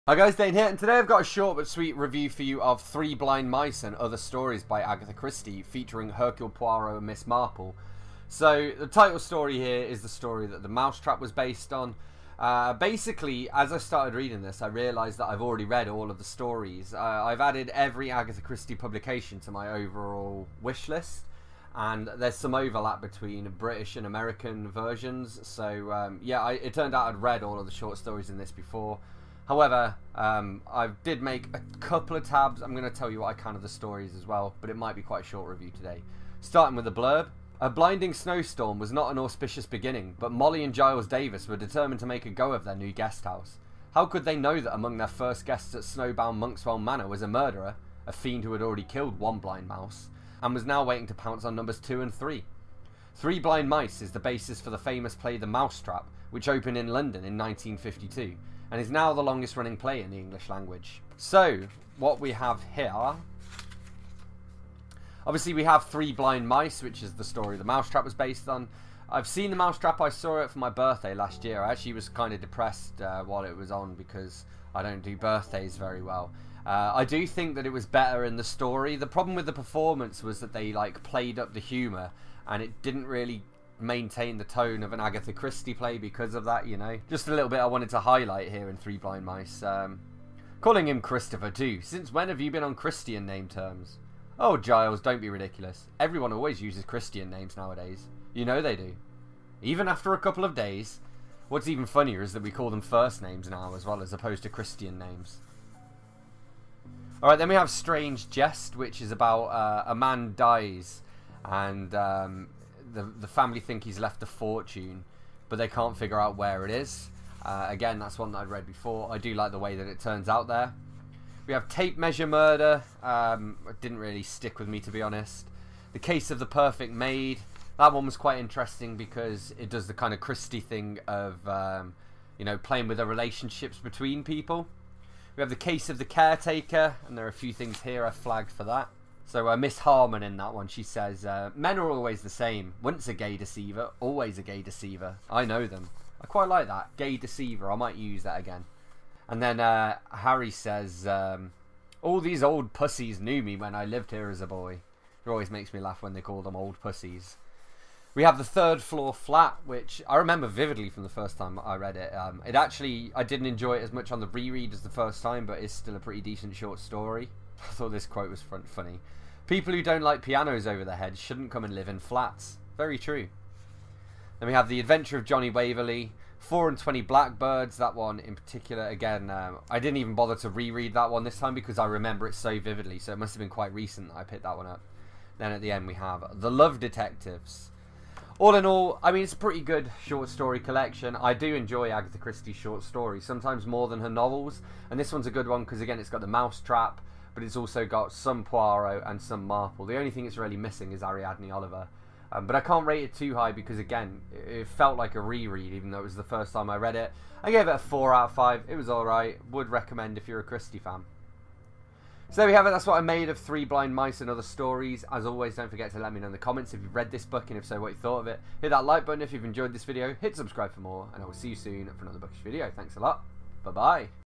Agatha Christie - Three Blind Mice and Other Stories [REVIEW/DISCUSSION]
A first time read that felt like a re-read.

BOOKS MENTIONED:
Agatha Christie - Three Blind Mice and Other Stories